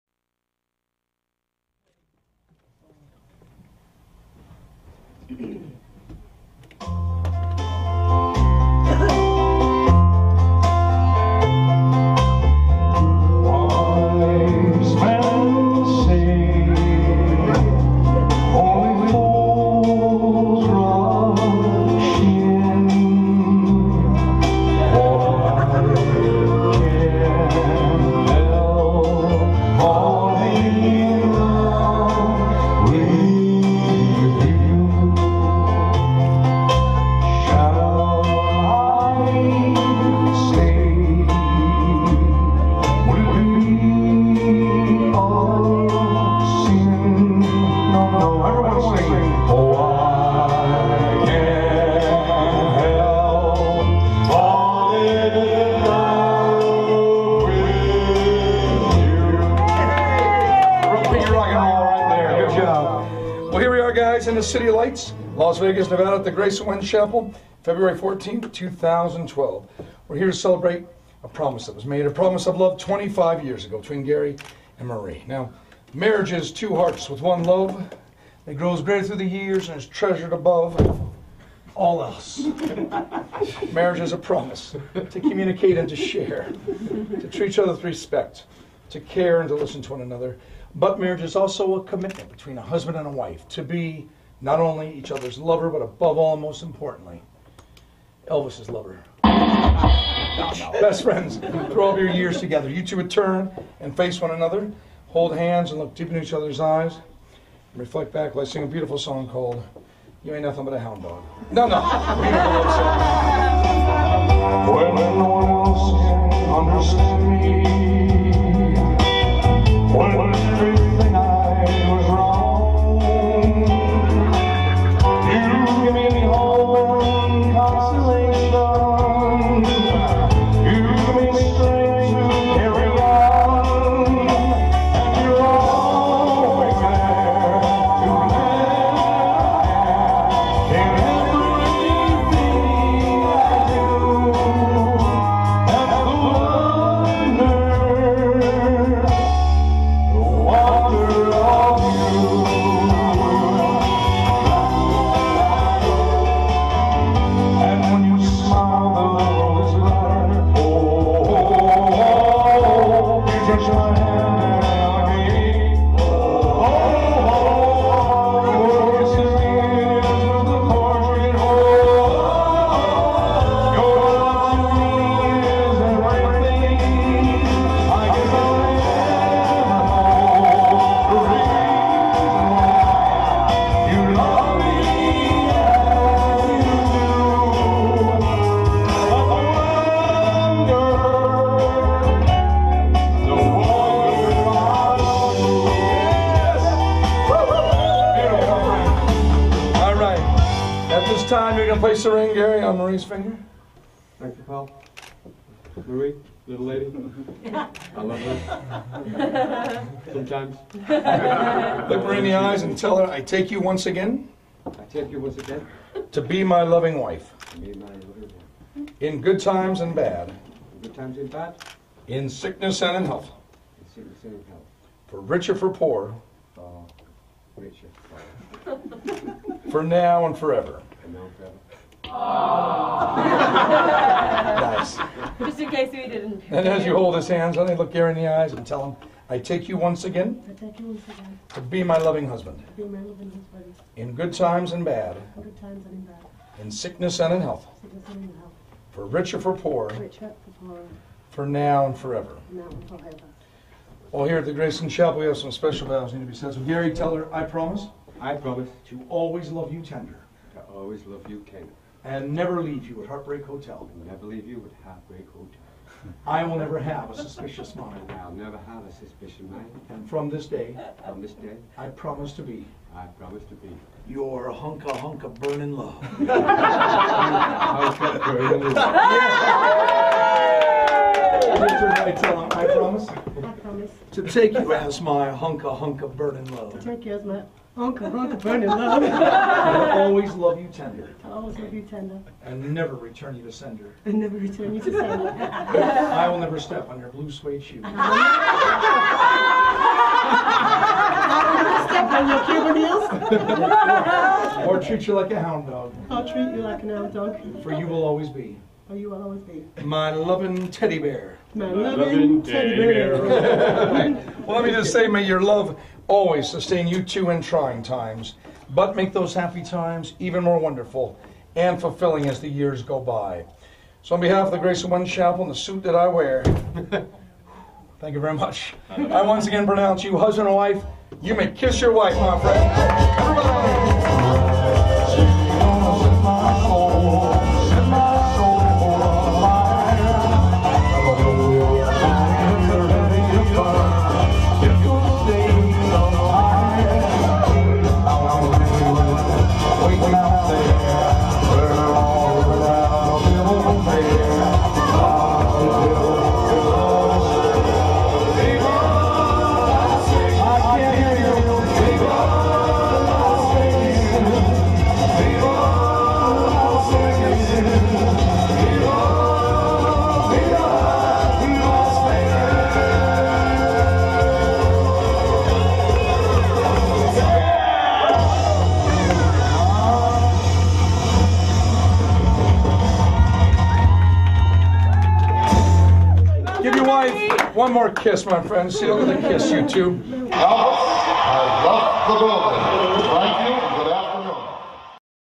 Gary and Maries 25th Wedding Anniversary Las Vegas.
Best Getaway Ever With The Greatest Bunch of Friends, February 14th 2012 .